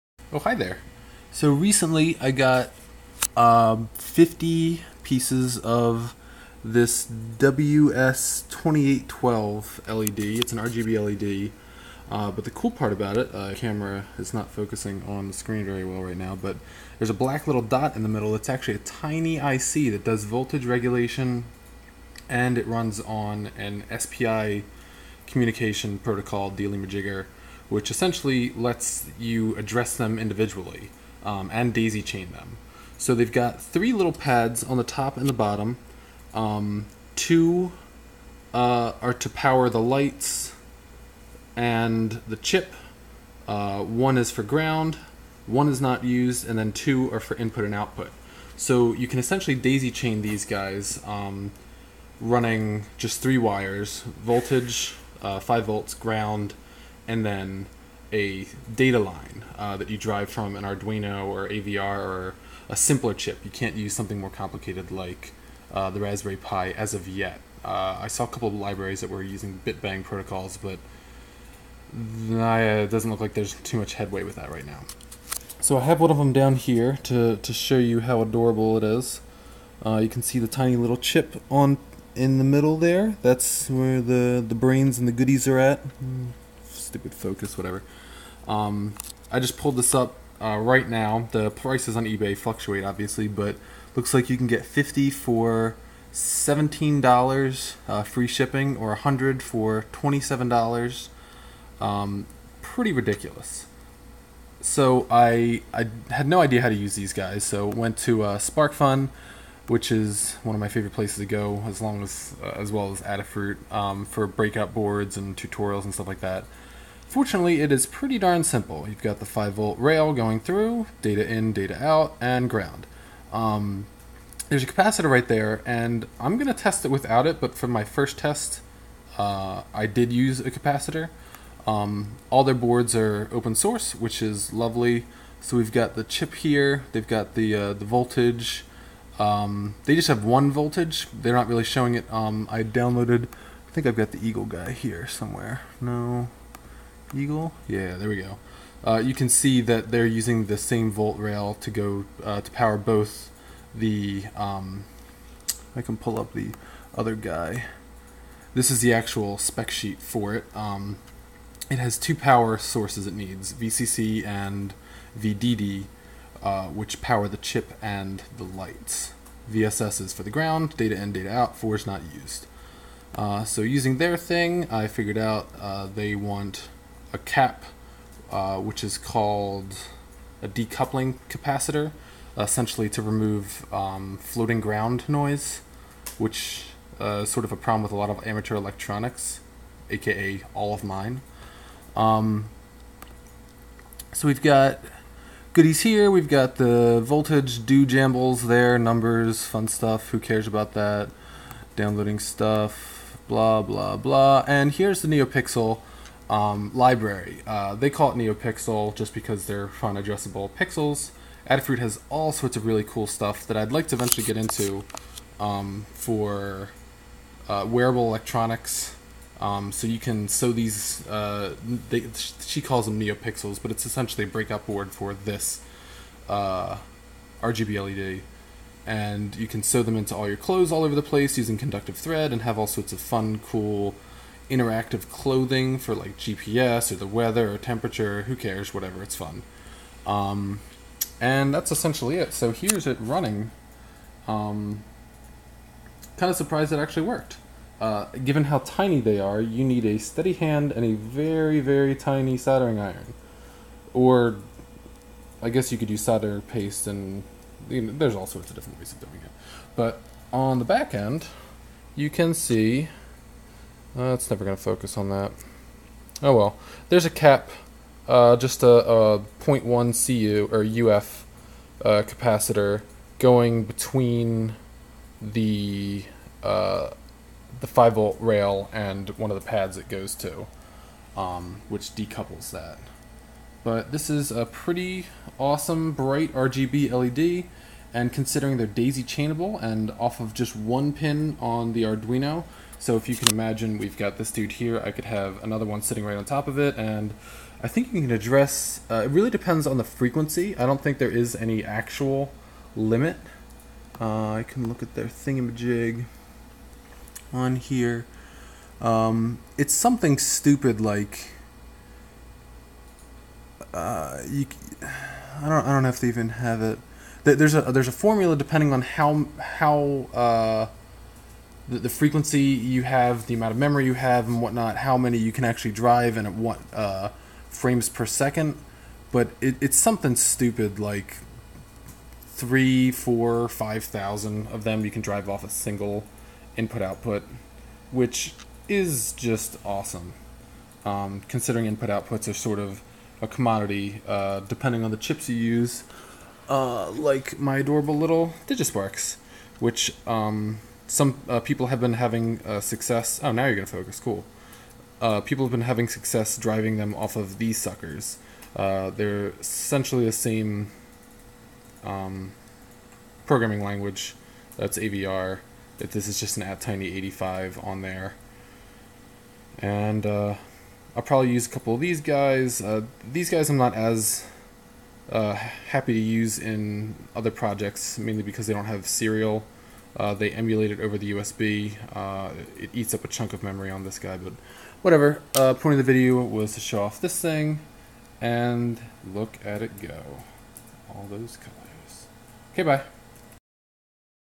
Using ws2812's for the first time
Made a quick video getting the ws2812's I just got working with arduino